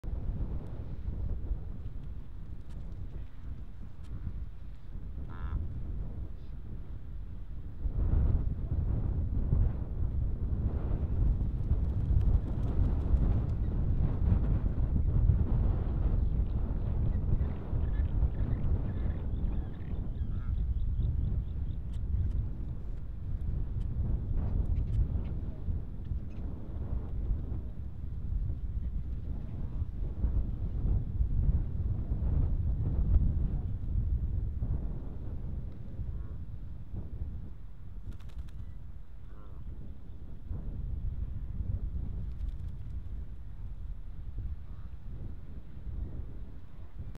Imperial Shag Courtship Behavior
These Imperial shags nest among the Rockhoppers in some of the penguin colonies along the rocky shore. Here, these birds are exhibiting courtship behavior at the start of the breeding season.

This video was taken at the Fanning Head Rockhopper rookery. Footage compiled in conjunction with work for the NGO, Falklands Conservation. for more info on current projects in the Falkland Islands.